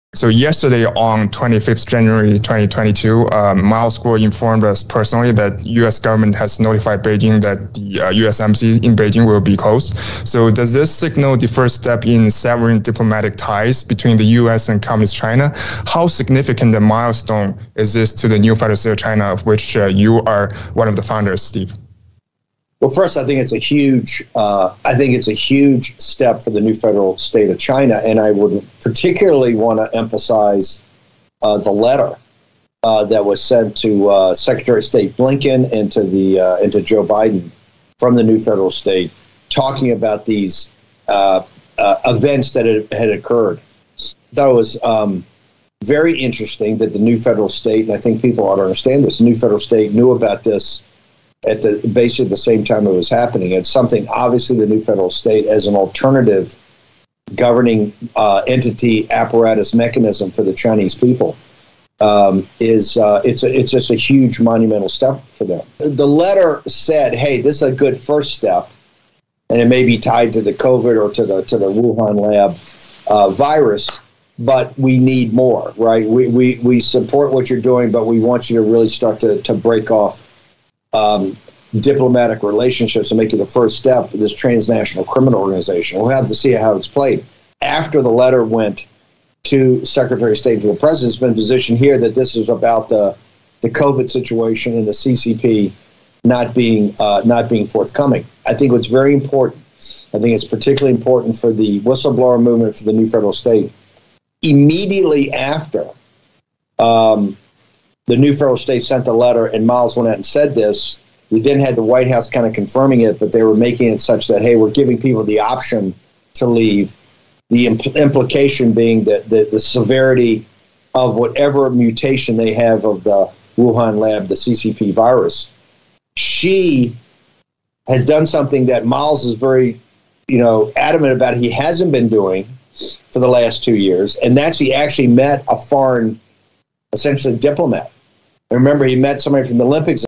班农附和郭骗中美即刻断交的谣言 美国政客的LOW出乎想象(2022.01.26)
作为一个老练的白宫旧幕僚 ，班农难道对中美即刻断交的谣言信以为真吗？班农知道附和郭骗谎言的价值。美国政客的LOW出乎想象，为了钱可以出卖灵魂，甚至为一张手绘的大饼就可以做资本家的泛走狗。